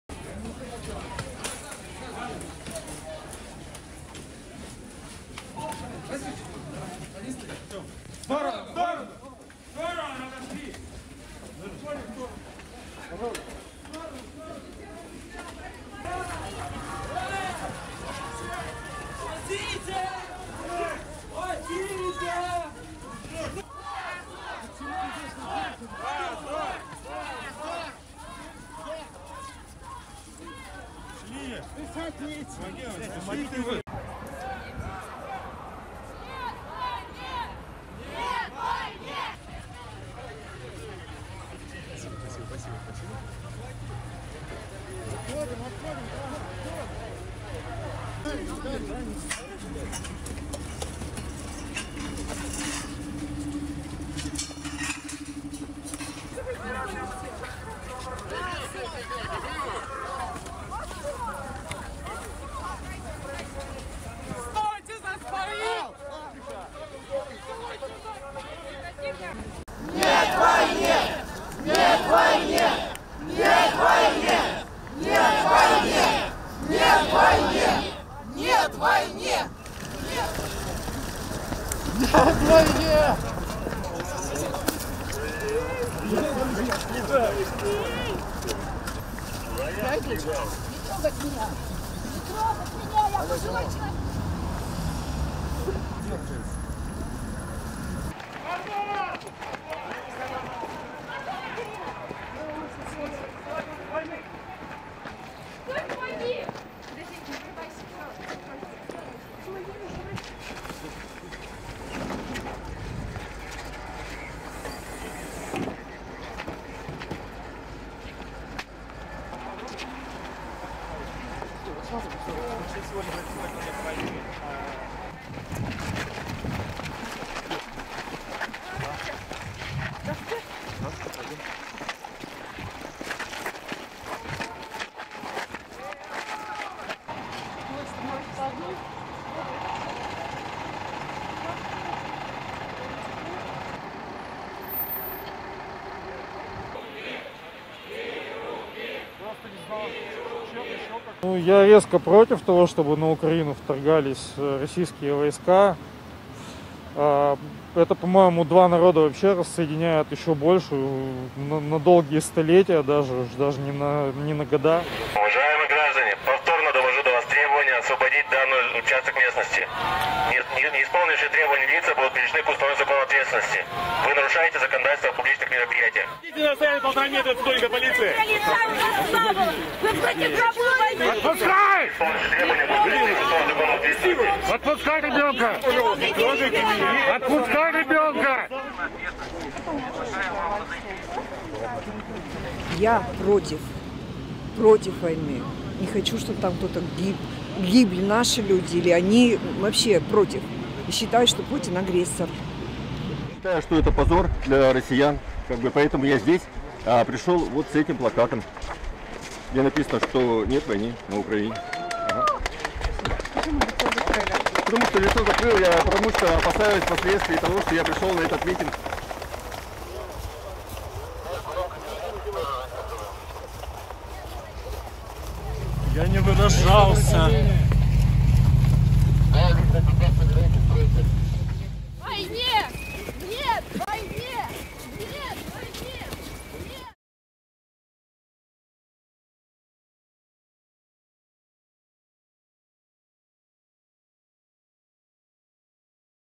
Hundreds arrested in anti war protests as Russians call on Putin to end war in Ukraine
this invasion has to stop